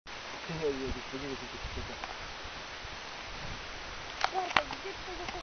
Buszujący w zbożu
Mistrz Burn pokonujący bezkresne górę zboża rowerkiem:)